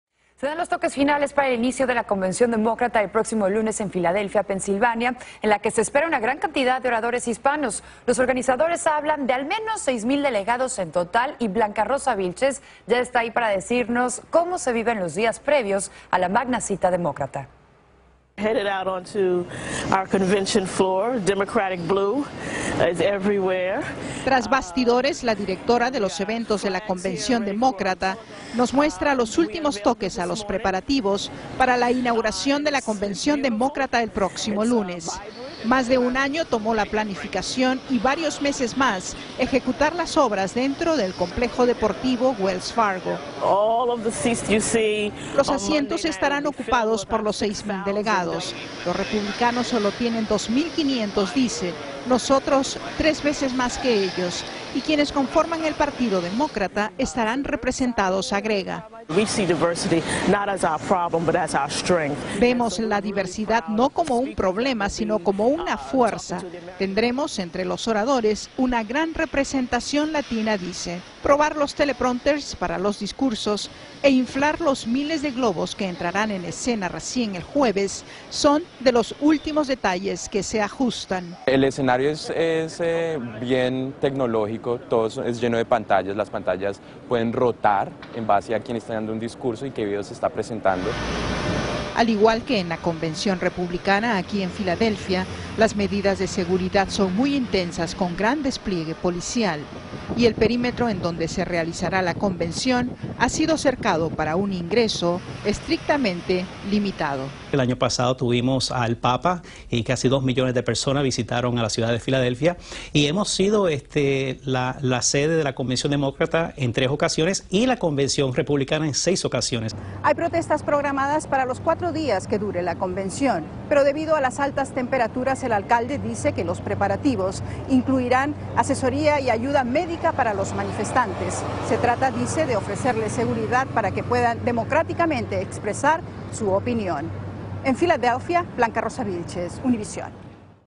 Finalizan detalles para la convención demócrata
Los preparativos se finalizan y se habla de al menos 6 mil delegados durante la convención demócrata que tardó más de un año para planificarse.